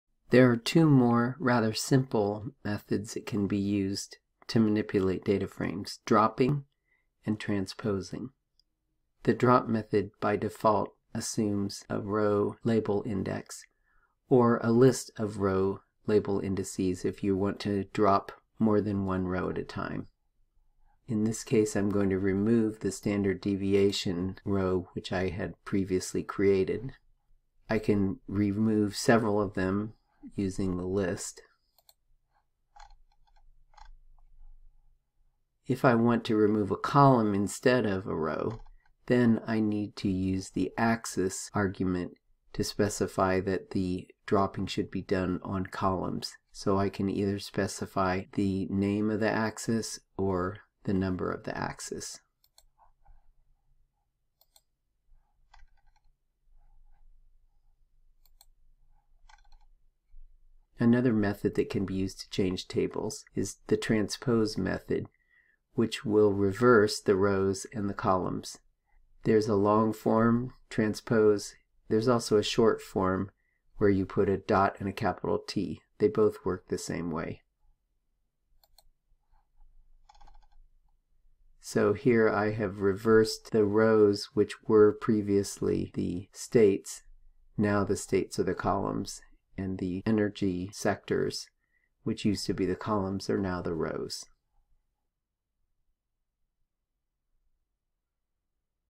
009ah Dropping and transposing in pandas DataFrames
Demonstrates how to remove a row or column in a pandas DataFrame and how to switch the rows and columns using the transpose method.